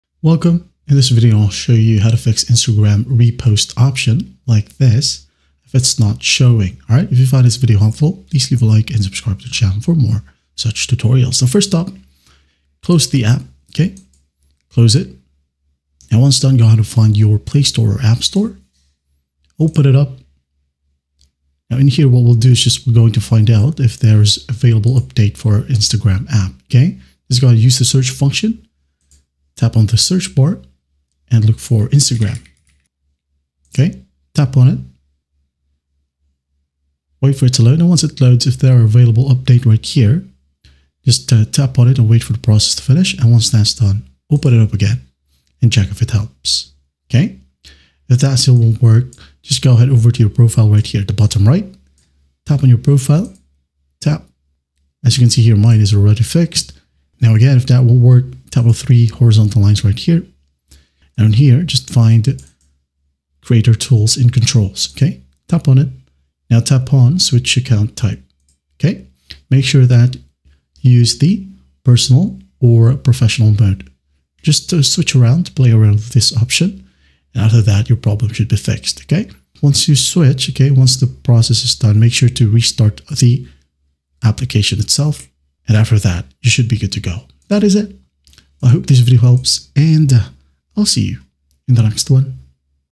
How to Fix Instagram Repost Option Not Showing (Easy Guide 2026)
How to Fix Instagram Repost Option Not Showing

In this video, I will show you how to fix the Instagram issue where the repost option is not showing on your account. This problem can happen if your app is outdated, the feature isn’t available in your region yet, or your account settings need adjustment. I’ll guide you through updating Instagram, checking feature availability, switching to a professional account, and using alternative repost methods. Whether you’re on iOS or Android, these solutions will help you get the repost feature back so you can share posts and reels with ease.

Chapters:
00:00 Introduction
00:10 How to Fix Instagram Repost Option Not Showing